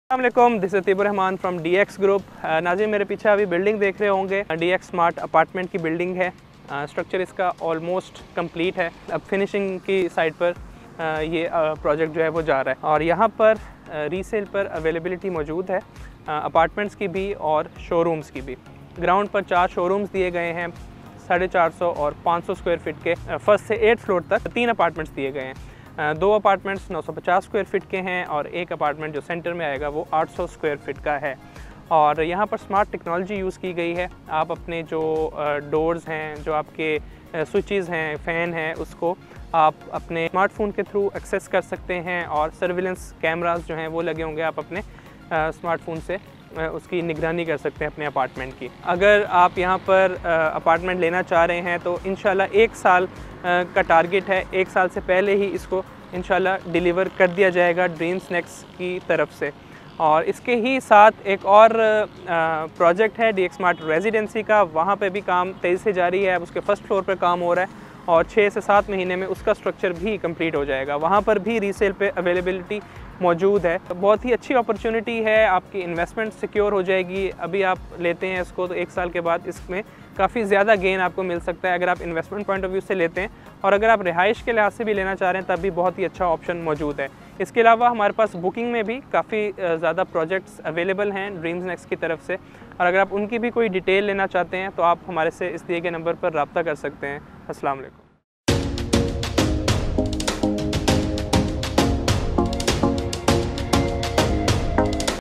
DX Smart Apartment - 2 Bedrooms & Resale Showrooms - Grey Structure Completed - Bahria Town Karachi
DX Smart Apartment - 2 Bedrooms - Grey Structure Completed - Resale Flat Available
Are you looking for a secure investment? The DX Smart Apartments is here now with the ground floor filled with showrooms and 3 different kinds of apartments on the rest of the floors. Advanced technology is also used to monitor your entire apartment and control it in the comfort of your room.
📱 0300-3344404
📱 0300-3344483